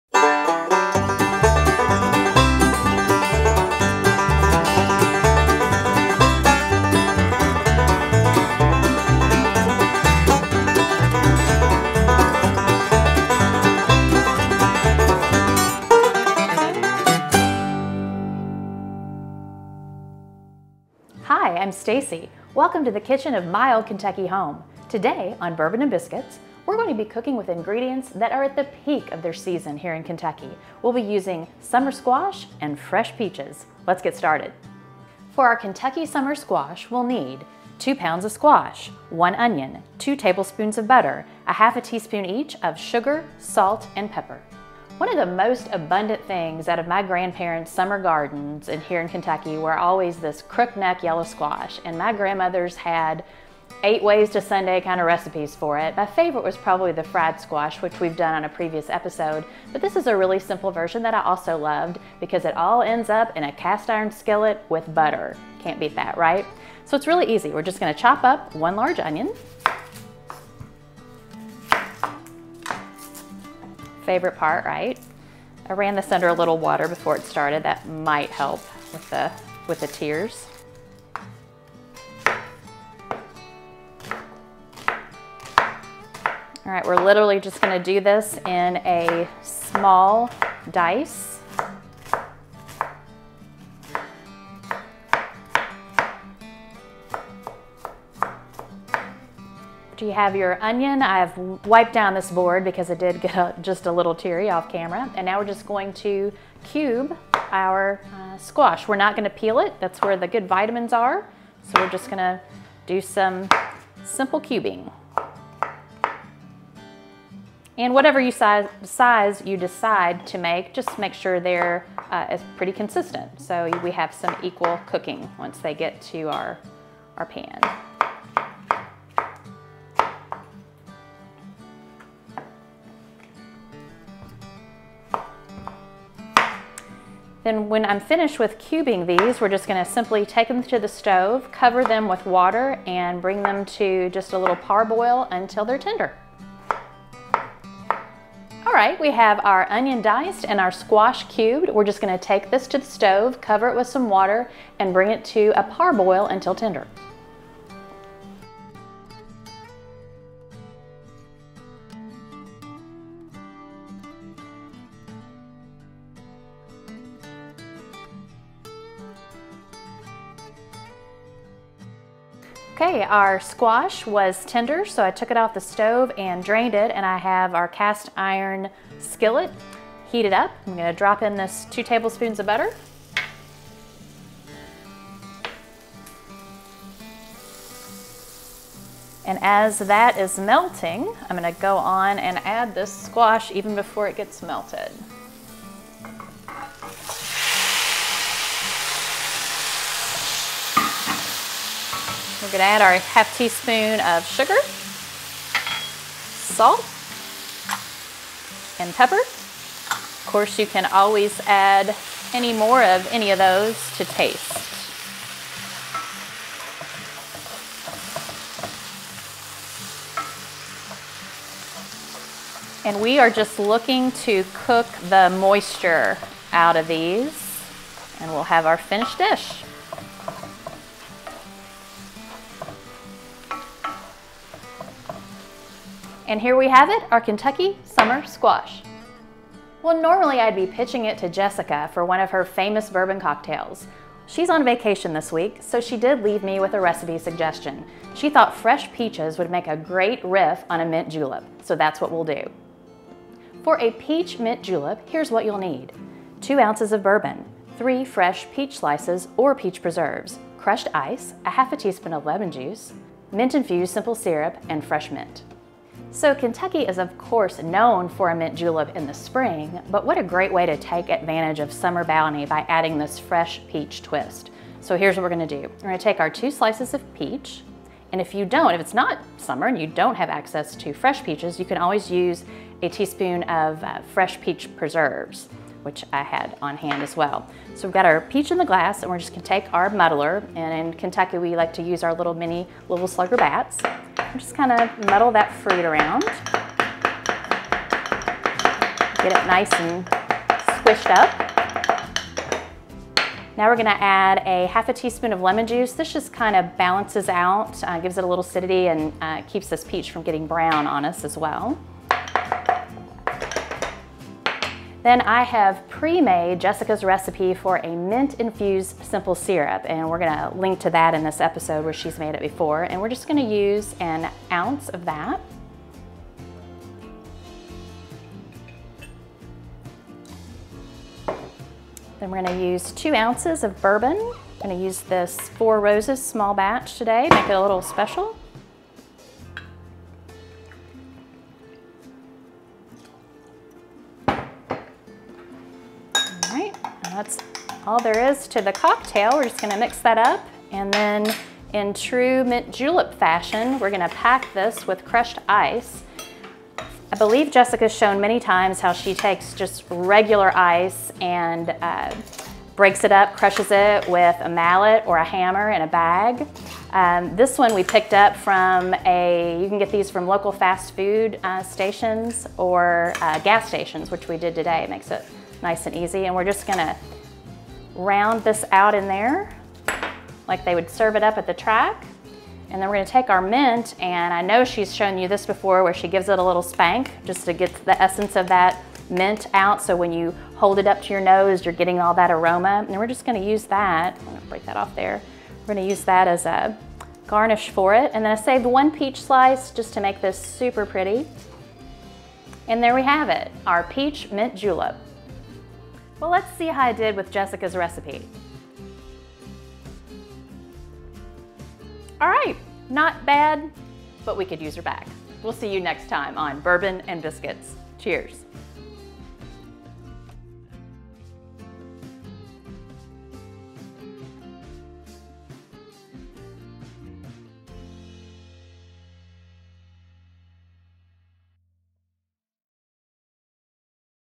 Kentucky Squash and Peach Mint Julep
In this episode Stacey and Jessica make Kentucky Squash and a Peach Mint Julep